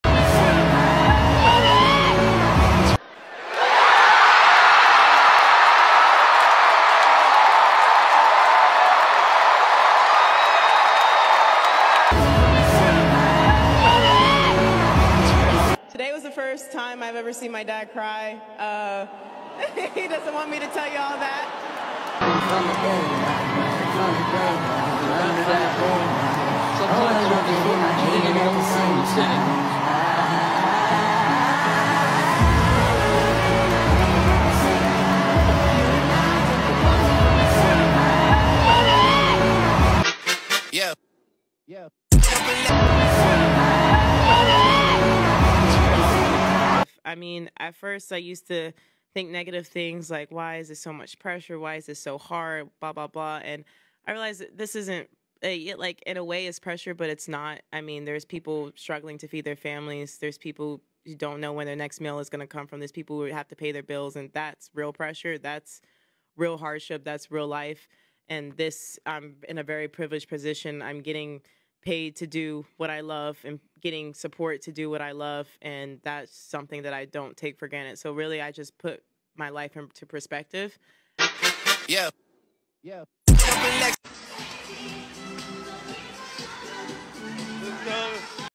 CocoGauff WINS THE USOPEN!!!
Coco Gauff is a poised rising star..
gives no excuses. just execute.
Great parenting also.